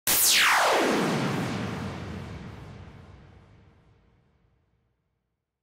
Falling Modular Laser (Omnisphere) | Splatoon Sound Sources
Ah, the good ol' days of Splatfest...

Used in:
- Splatfest - Opening (S1)

If you want more information on these, check out our document list of every Splatoon findings we've concocted
You can also join this... weird-shaped blue logo (otherwise known as "Discord") server so you can keep up with our latest findings

That's all from us. I'm gonna go do some Splatfest shenanigans with the interns.
As always, "stay fresh"!

Sample found by: Natah/Luna (SeaNothing)
Demo by: Natah/Luna (SeaNothing)

- SRL Intern